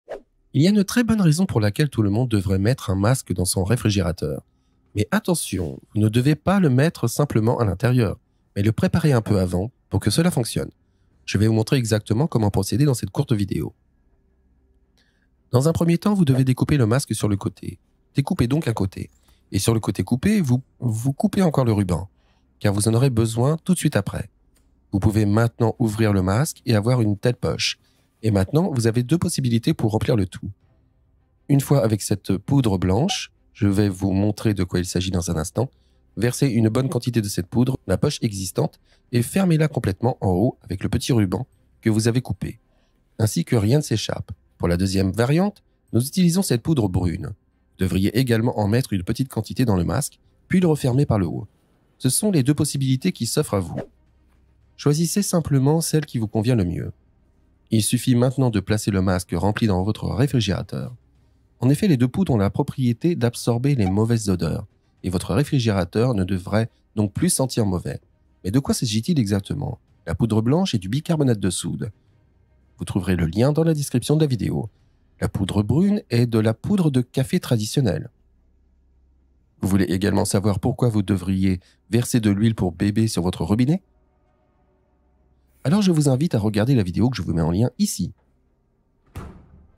Accrochez votre masque dans le frigo et SOYEZ SURPRIS DU RÉSULTAT🤯💥
As-tu déjà mis un protège-dents dans ton réfrigérateur ? Je te montre ici ce que cette astuce géniale peut t'apporter !

Tu as certainement aussi quelques protège-dents chez toi. Au lieu de les laisser simplement dans le placard, je te montre dans cette vidéo une façon géniale d'utiliser un protège-dents dans ton réfrigérateur.

Tout ce dont tu as besoin est le protège-dents et un peu de bicarbonate de soude. Découpe le protège-dents sur le côté et verse ensuite un peu de bicarbonate de soude dans celui-ci. Avec le ruban du côté coupé, tu peux maintenant refermer le protège-dents.

Tu peux ensuite le placer dans ton réfrigérateur. Le bicarbonate de soude absorbe les mauvaises odeurs et grâce au protège-dents, tu peux le placer dans le réfrigérateur et veiller à ce que les mauvaises odeurs soient éliminées.

Si tu as encore un protège-dents, n'hésite pas à essayer cette astuce.